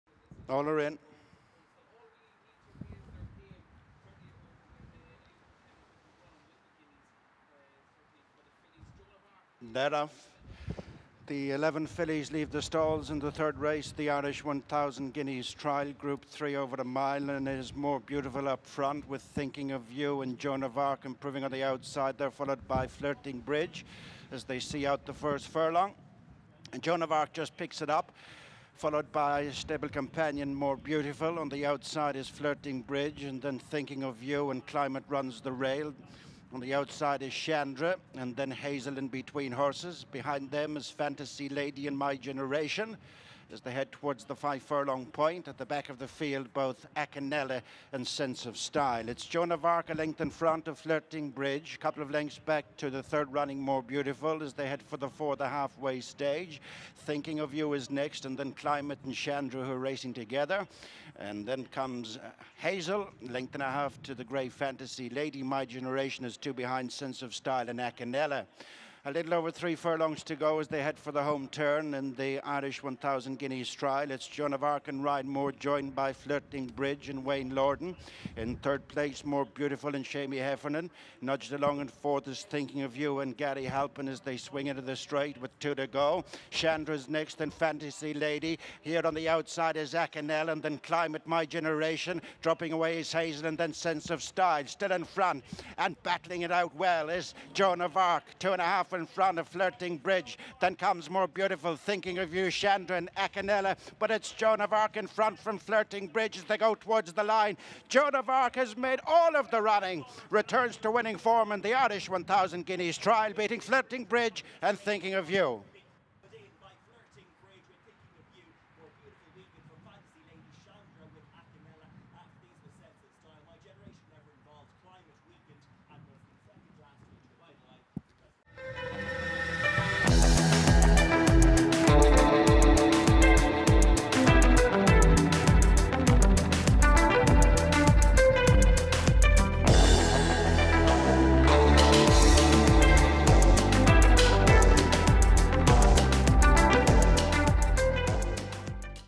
JOAN OF ARC wins the Irish 1,000 Guineas Trial (Group 3) at Leopardstown under Ryan Moore
Aidan O’Brien’s Joan Of Arc dominated the 2021 renewal of the Irish 1,000 Guineas Trial at Leopardstown. The 11-4 favourite, ridden by Ryan Moore, was in front after a furlong and stayed there without ever having to come under strong pressure.

Up to a mile in this Group Three, the daughter of Galileo – a full-sister to Classic winners Gleneagles and Marvellous – always appeared in control and had a length and three-quarters in hand at the line. Her nearest pursuer was 50-1 shot Flirting Bridge, with the same distance back to another outsider Thinking Of You (40-1) in third.